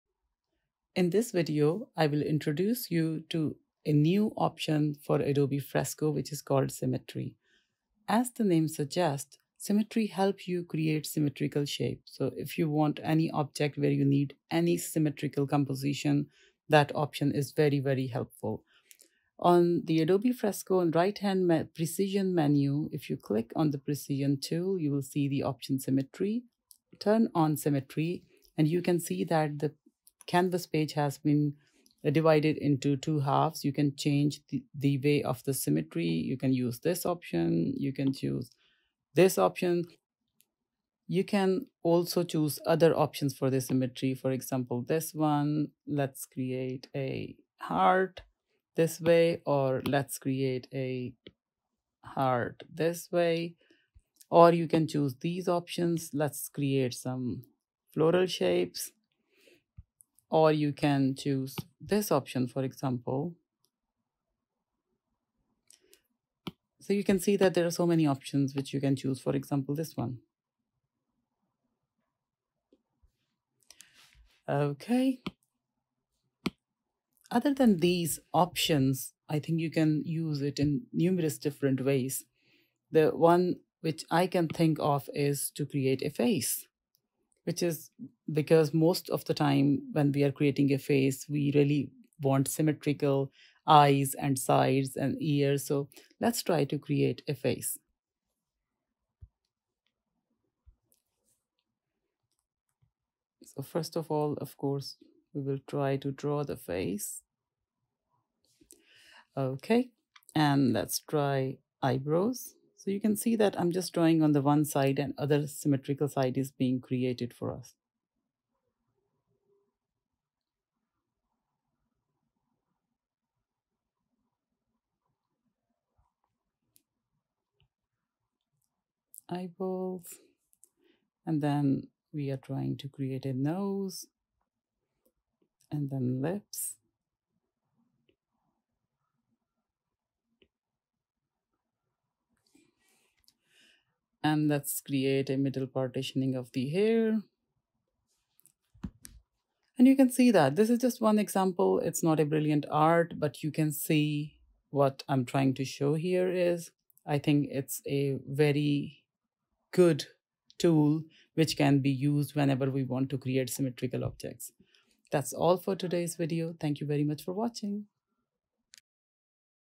Create A Face using Symmetry Feature - Adobe Fresco
This video explains new feature of Adobe Fresco called 'symmetry' which is a very handy tool to create symmetrical objects.